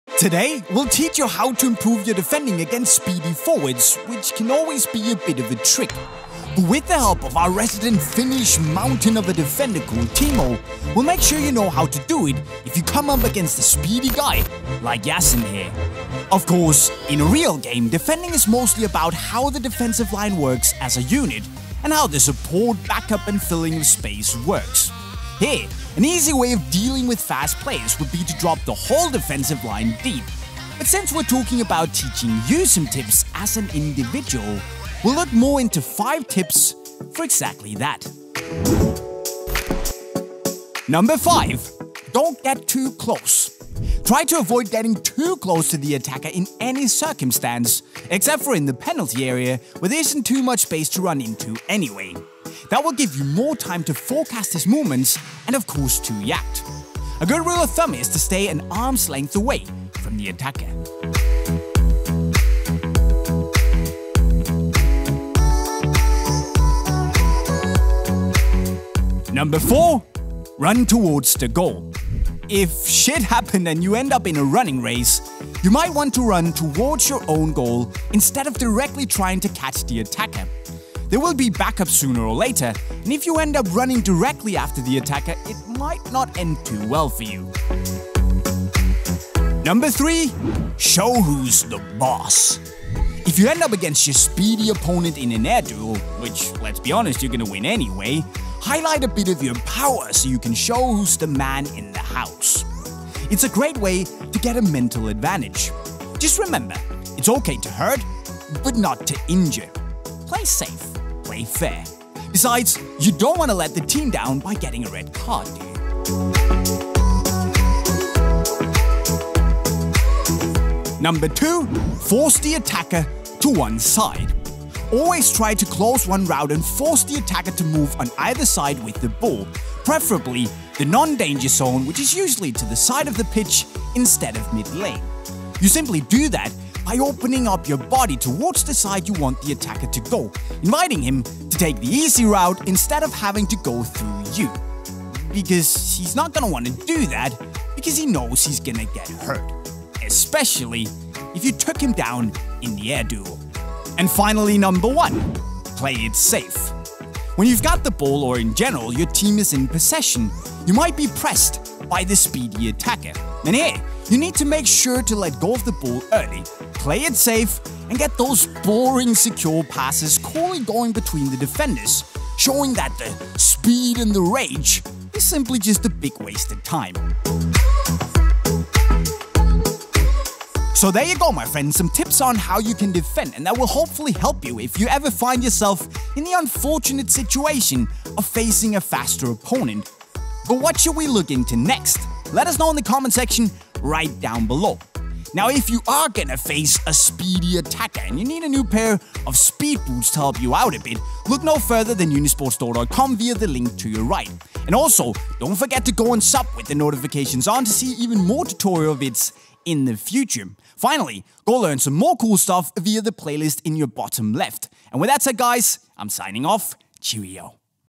Learn how to defend against speedy forwards
How to defend against speedy attackers - in today's video, we will teach you how to learn to defend against fast attackers by giving you five pieces of advice on how to defend on the pitch against a speedy player. It's not always easy to defend when your opponent is really fast, and if you want to learn how to improve your chances of defending well against the attacker, we have five great tips for you in today's video, where our resident Finnish defending rock, Timo, will give you a few good pieces of advice on how to make sure you will always defend well against attackers with speed in order to not look stupid if they're too fast to catch. Also, remember to work closely together with the rest of the defending line.